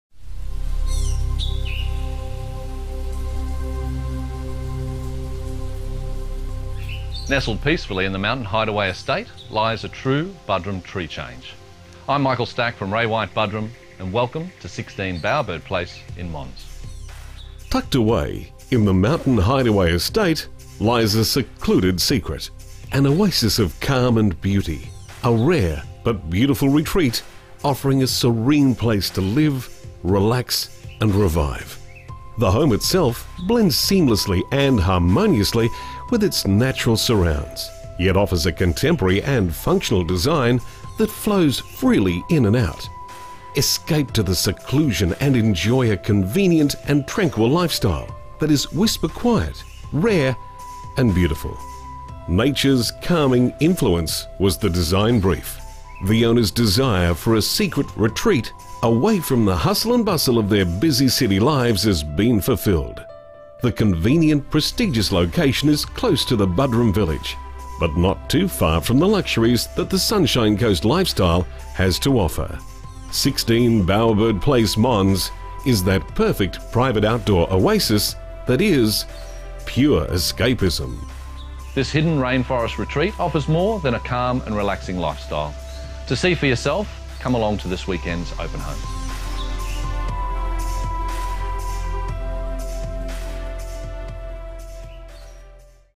SOLD - 16 Bowerbird Place, Mons Mountain Hideaway Estate by Michael Stack Ray White Buderim
An Oasis of Calm and Beauty

Tucked away in the Mountain Hideaway Estate lies a rare but beautiful retreat offering a serene space to live, relax and revive. The home itself blends seamlessly and harmoniously with its natural surrounds yet offers a contemporary and functional design that flows freely in and out. Escape to the seclusion and enjoy a convenient and tranquil lifestyle that is whisper quiet, rare and beautiful.

Natures calming influence was the design brief, the owners wanted a secret retreat away from the hustle and bustle of their busy city lives, it had to be located in a convenient but prestigious location, close to the Buderim Village and not far from the luxuries that the Sunshine Coast lifestyle offers.

Well can i say they certainly achieved their dream, their idyllic secluded secret is now out and they want everyone to know about it. If you're serious about privacy and you love everything that mother nature has to offer then 16 Bowerbird Place is the perfect private outdoor oasis that is pure escapism.